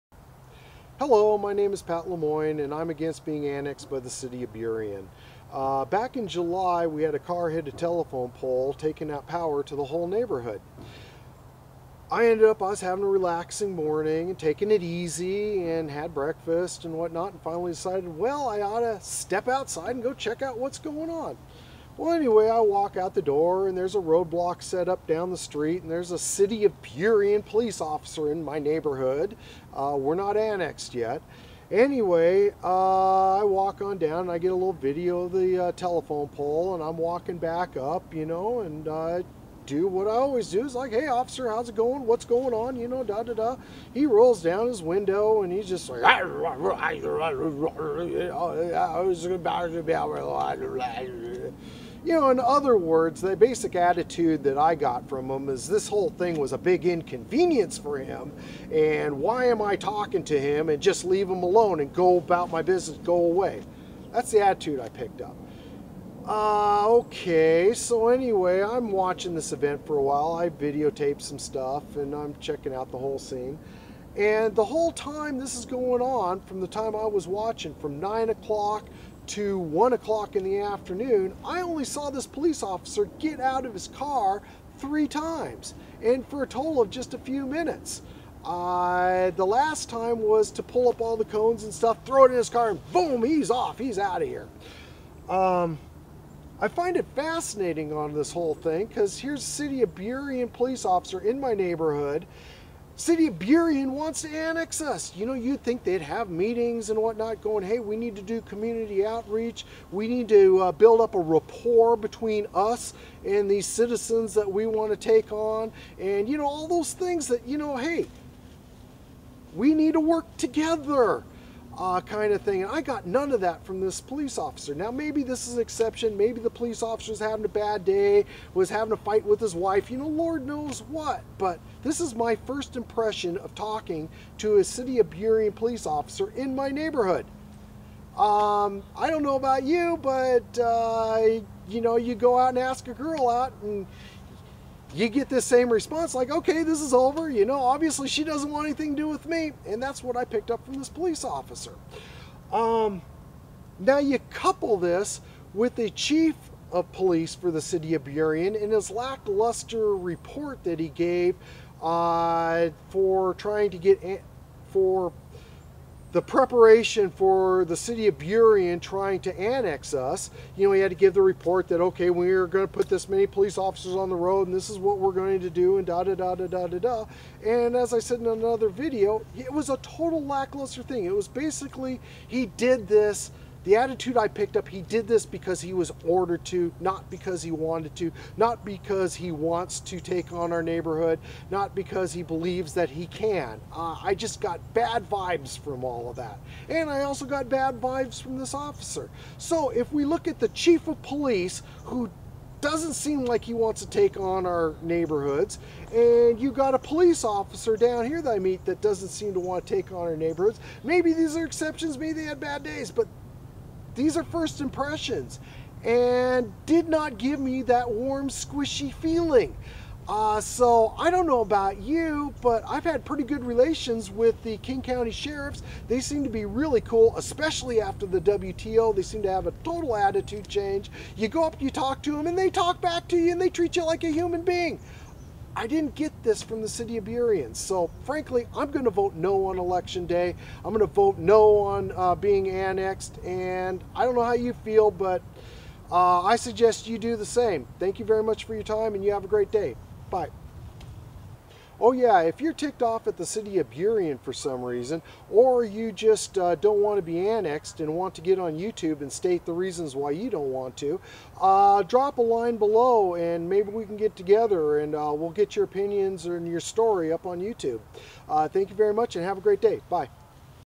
Annexation Bad Idea #13 Warm Squishy Feeling Burien WA
I don't think the Burien police want to patrol our neighborhoods.  I base this on the Burien Chief of Police in a speech given several months ago and the attitude of a Burien police officer in my neighborhood.